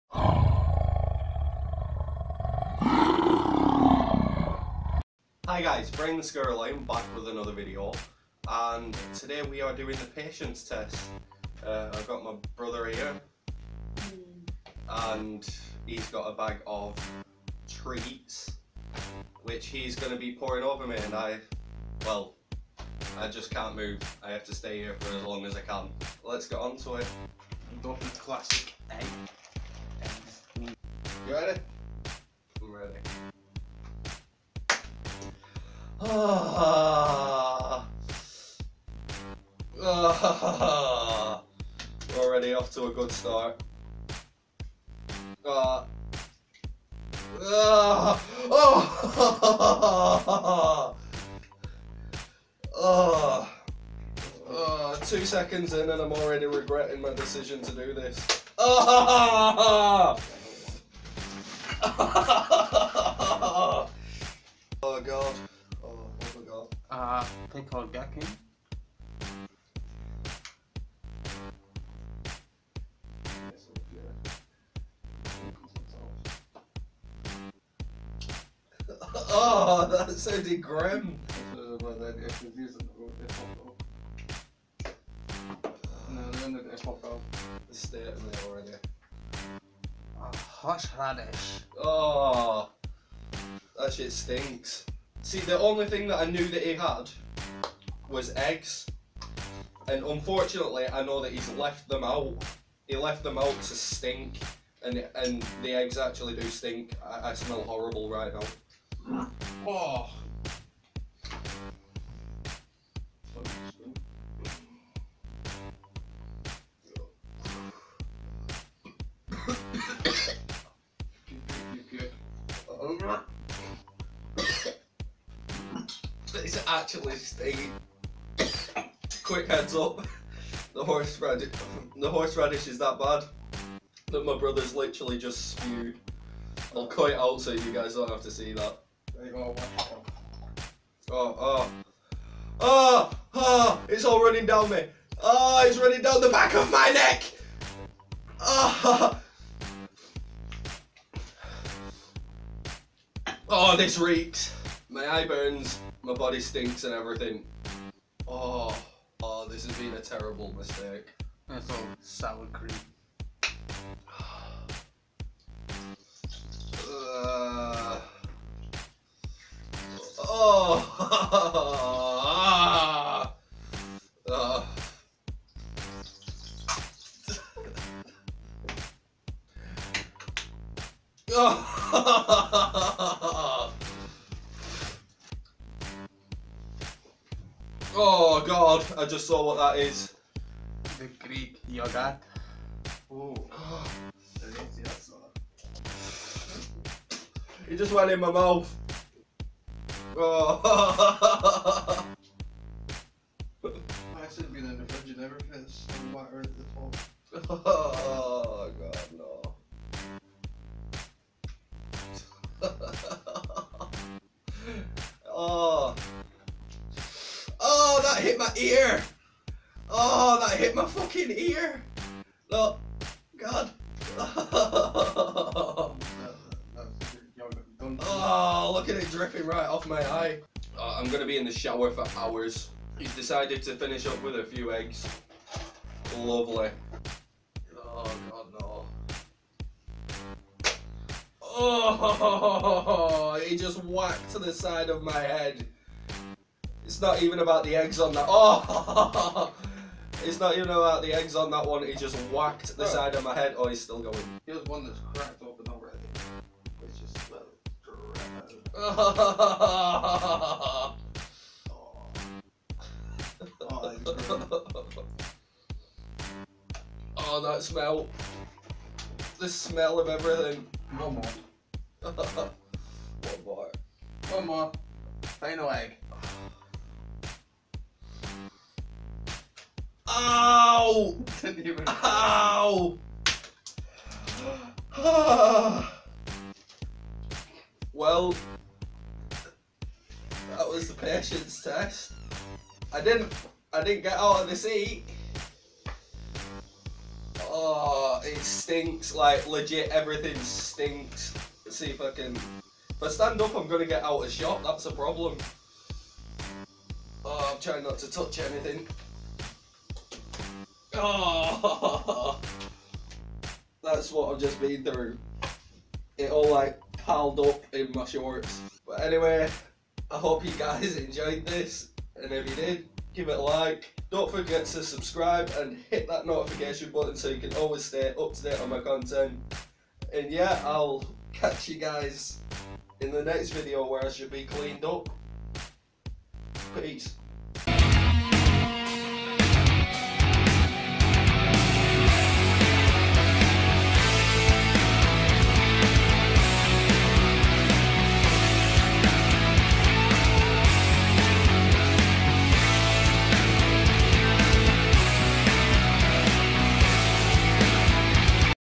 Very Eggy Patience Test
So i decided to let my brother pelt me with different things for a patience test and I can honestly say I never want to go through this again.